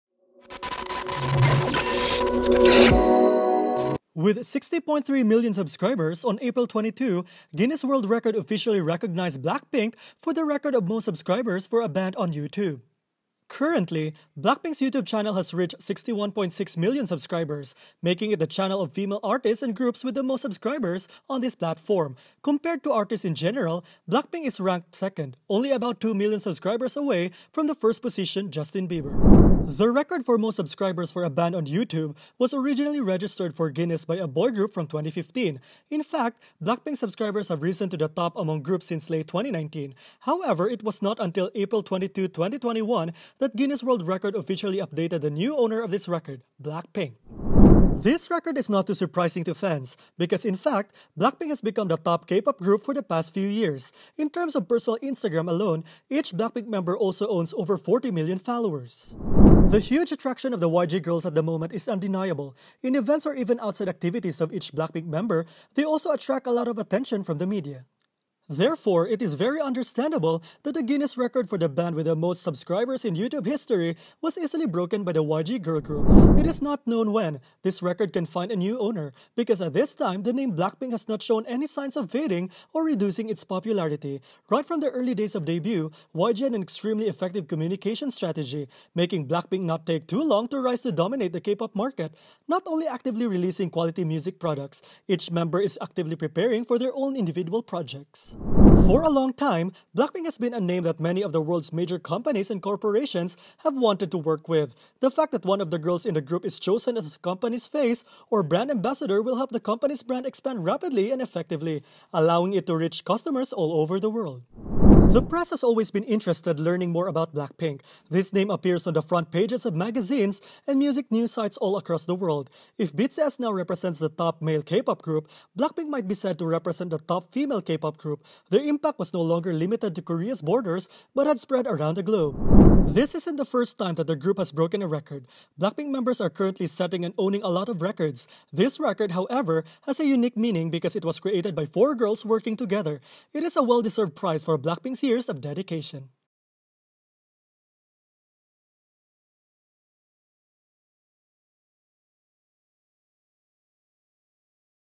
BLACKPINK sets a Guinness record as the group with the most YouTube subscribers
With 60.3 million subscribers, on April 22, Guinness World Record officially recognized BLACKPINK for the record of "Most subscribers for a band on YouTube".